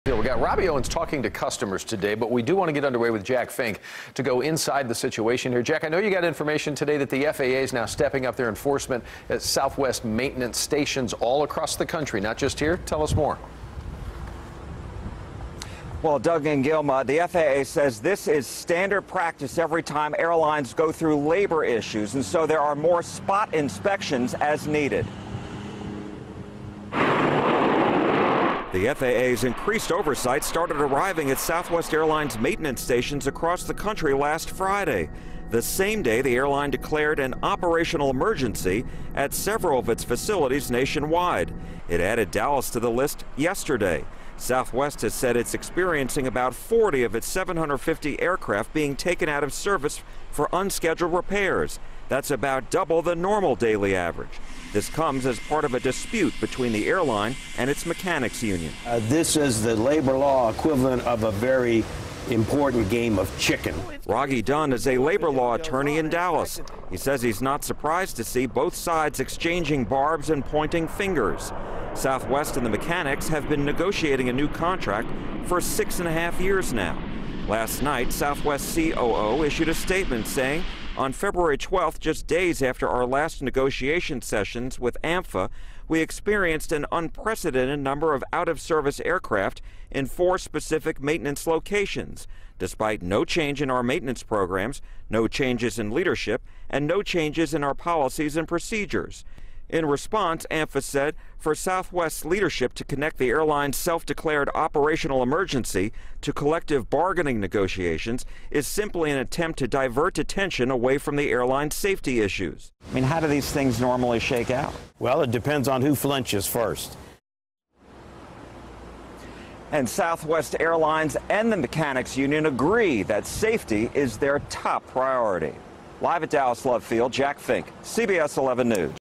FAA Steps Up Enforcement At Southwest's Airlines Maintenance Stations Nationwide
The FAA says it's standard practice when airlines are working through labor issues. Both Southwest and its mechanics union insist safety is their top priority.